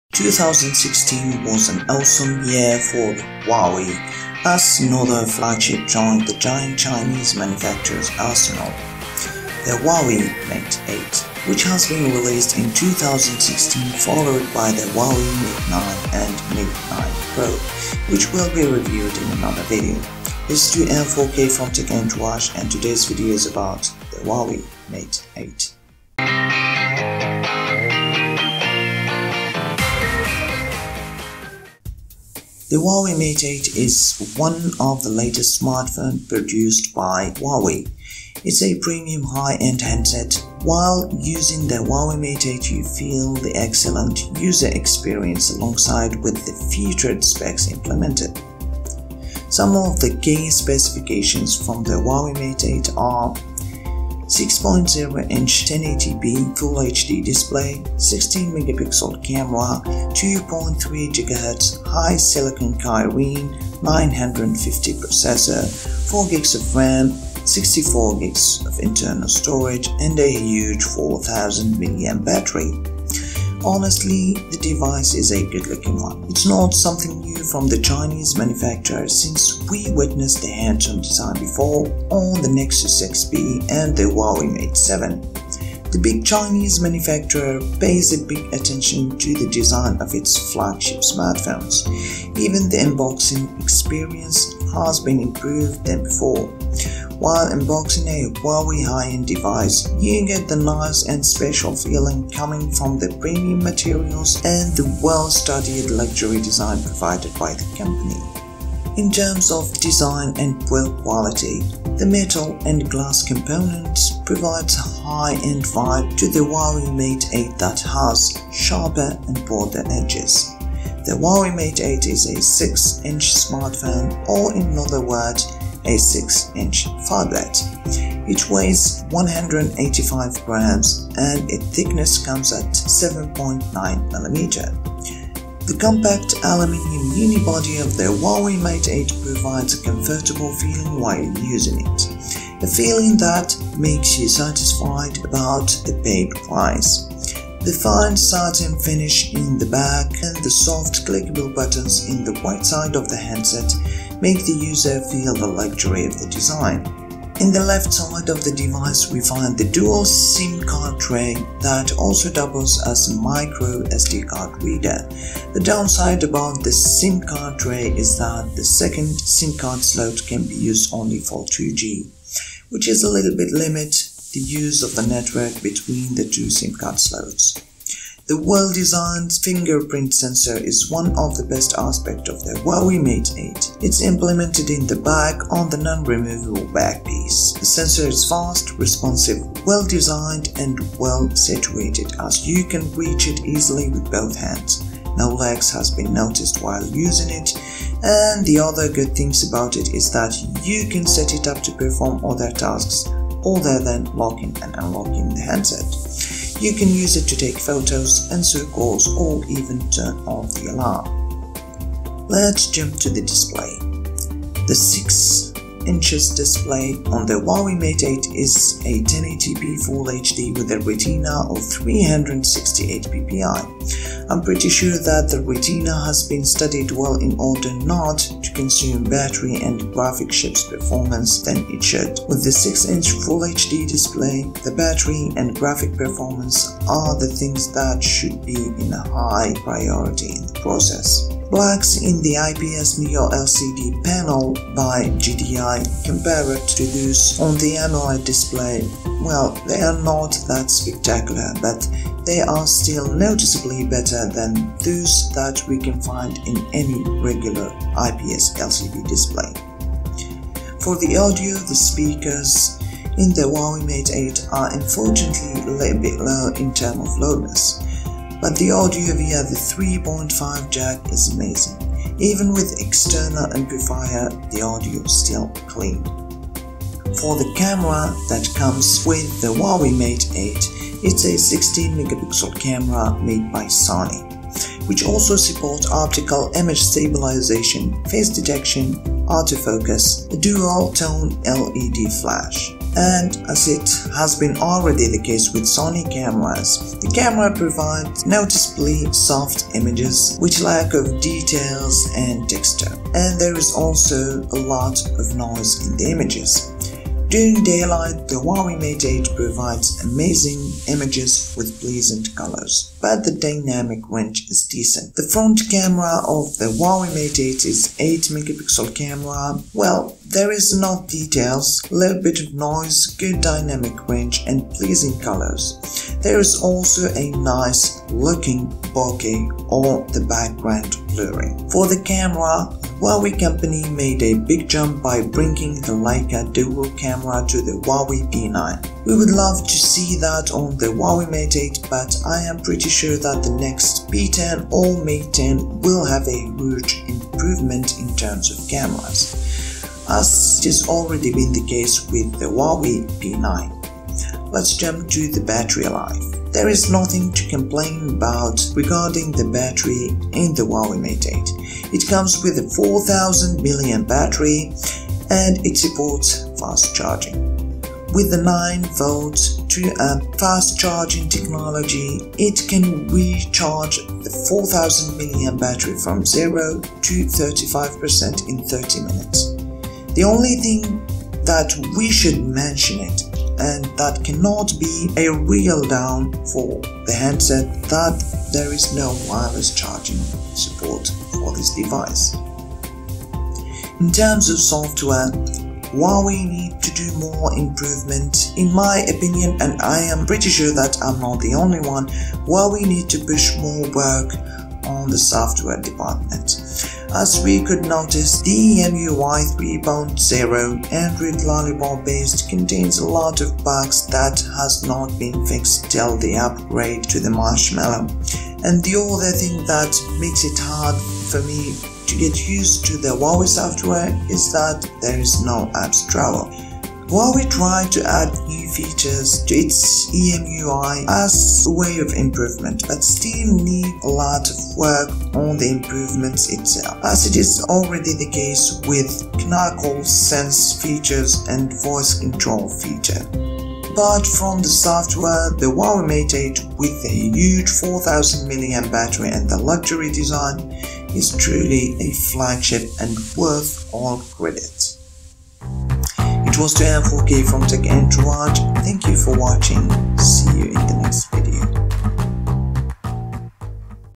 Huawei Mate 8 Hands On
2016 was an awesome year for Huawei as Another flagship joined the giant Chinese manufacturer arsenal , the Huawei mate 8 , which has been released in 2016 followed by the Huawei mate 9 and mate 9 pro which will be reviewed in another video ,

This is 2M4K from tech entourage  and todays video is about the  Huawei mate 8

Introduction
The Huawei Mate 8 is one of latest smartphones produced by Huawei . It's a premium high end handset, while using the Huawei mate 8  you feel the excellent user experience alongside with featured specs implemented.
The Key specifications for the Huawei mate 8 are :
6.0"1080x1920 pixels
16MP1080p
2.3GHzHiSilicon Kirin 950
4GB RAM64GB memory
4000mAhLi-Po
Honestly the device is a good looking one, it is not something new from the Chinese manufacturer , since we witnessed the handsome design before on the NEXUS 6P and Huawei mate 7 , the big Chinese manufacturer pays a big attention to the design of its flagship smartphones, even the unboxing experience has been improved than before , while unboxing Huawei high end device you get the nice and special feeling coming from the premium materials and well-studied luxury design provided by  the company .
The metal and glass components provides a high-end vibe ,The Mate 8 has a sharper and bolder edges.
The Huawei Mate 8 is a 6-inch smartphone. Or in other words 6 inch phablet .

the Weight is : 185g

and the Thickness is  7.9mm
the compact aluminium uni body of the huawei mate 8 provides comfortable feeling while using it, a feeling that make you satisfied about the payed price.
The fine satin finish in the back and the soft clickable buttons in the right side of the handset make the user feel the luxury of the design .

In the left side of the device we find the Dual SIM card tray, that also doubles as a micro SD card reader , the down side about the Sim Card tray is that the second Sim card slot can be used only for 2G , which a little bit limit the use of the network between the 2 sim card slots
The well designed fingerprint sensor , is one of the best aspects of the Huawei mate 8 , it is implemented in back on the non removable back peace . the sensor is fast, responsive, well designed , and well situated , as you can reach it easily with both hands . no lags has been noticed while using it . and the good thing about it is that you can also set it up to perform other tasks than just locking/unlocking the handset , you can use it to take photos, answer calls, or even turn off the alarm .

Display
The 6-inch display on Huawei Mate 8 is  1080p  FullHD ,with a retina of  368ppi, I am pretty sure that the retina has been studied well in order not to consume battery and graphic chip performance than it should  , with the 6 inch full hd display , this  battery and graphic performance are things that should have a high priority in the process .
Blacks in the IPS-NEO LCD panel by JDI,  compared to those on an AMOLED display, are not that spectacular but they are still noticeably better than those  on any regular IPS LCD.

Audio

The speakers in the Huawei mate 8 are a little bit low in terms of loudness , but the audio via the 3.5 jack is amazing.
Even with external amplifier the audio still clean .